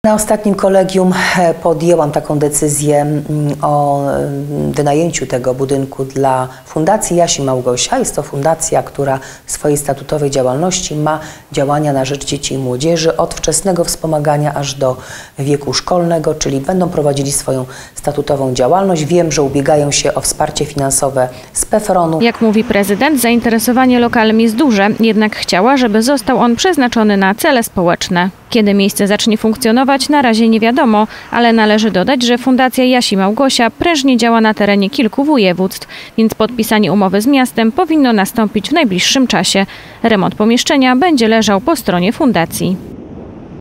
FUNDACJA „JAŚ I MAŁGOSIA” WYNAJMIE POMIESZCZENIE, W KTÓRYM MIAŁ POWSTAĆ SKLEP SOCJALNY
Pierwotnie, w lokalu przy ulicy Koszalińskiej miał powstać sklep socjalny, który planowała otworzyć Fundacja „Wolne Miejsce”. Niestety koszt powstania takiego miejsca wiązał się ze zbyt wysokimi kosztami, więc lokal pozostawał niezagospodarowany i po prostu niszczał. Teraz, Prezydent Anna Mieczkowska postanowiła co z tym miejscem zrobić.